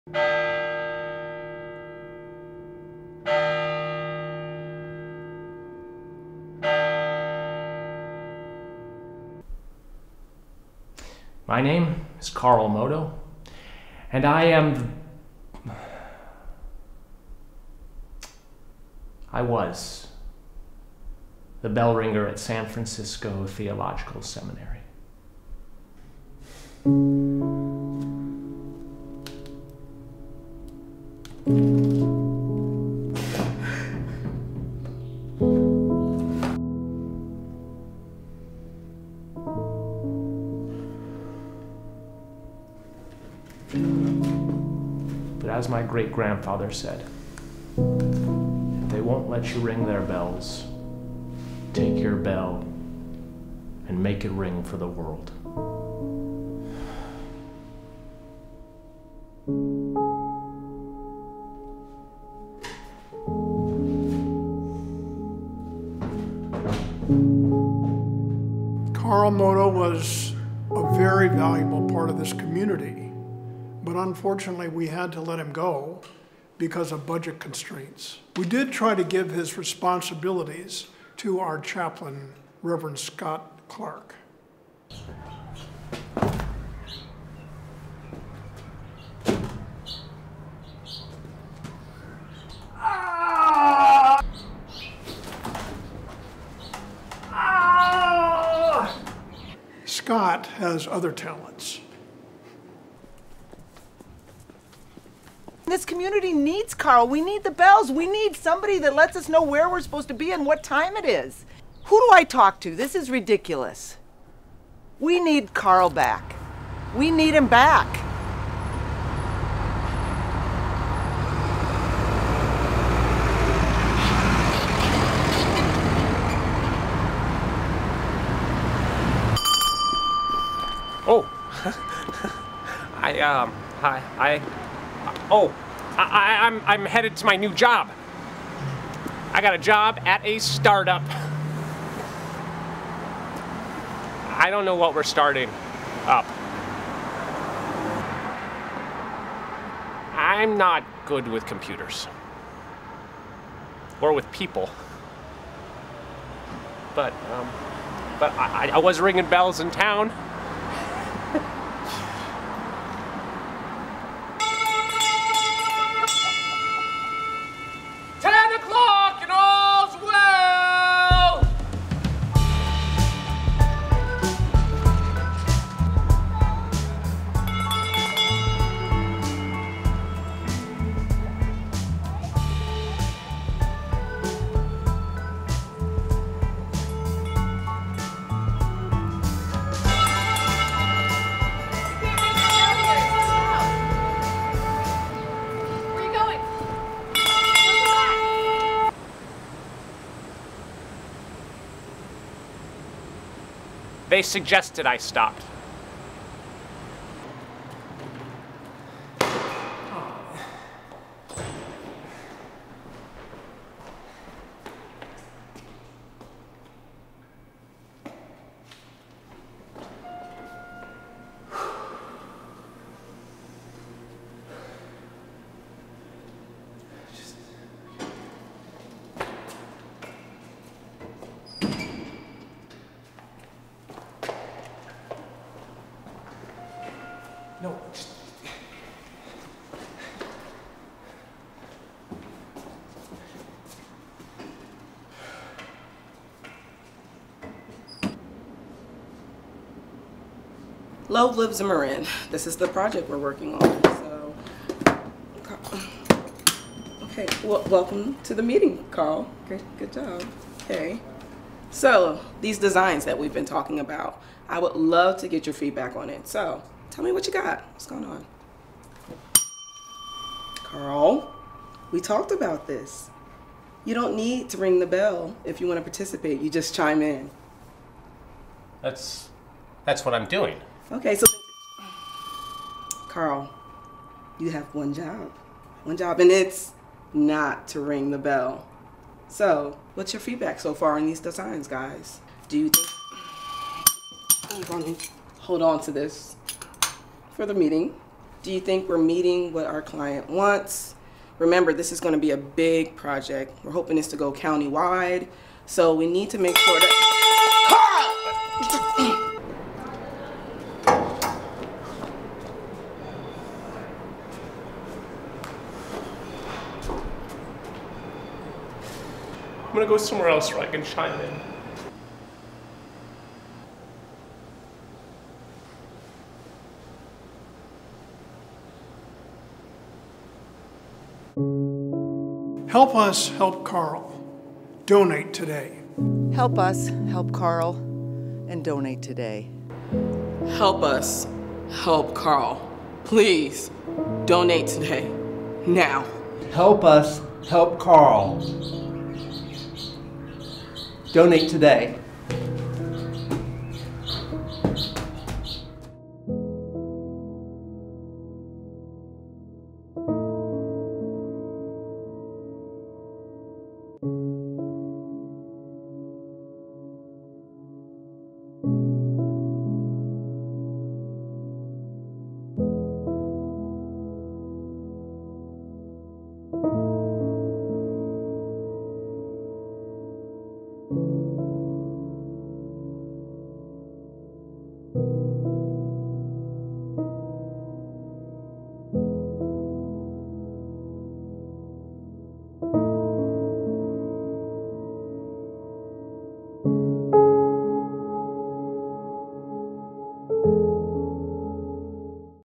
Help Us Help Carl: Bring Back the Seminary Bells!
Meet Carl Modo
Carl Modo is the seminary's bell ringer. He has been the seminary's bell ringer since he learned to ring the bells at his grandfather's knee, and comes from a long line of seminary bell ringers. Sadly, due to budget cuts, the seminary has had to let Carl go. Unfortunately, all Carl knows how to do is ring the bells at San Francisco Theological Seminary, yet he must venture into the world and find work elsewhere. As you'll learn in the entirely student-produced viral video, Carl's, um, special talents don't quite work in the outside world! The seminary needs to get Carl back, and the only way we can do that is with your help! Give Today: sfts.edu/bells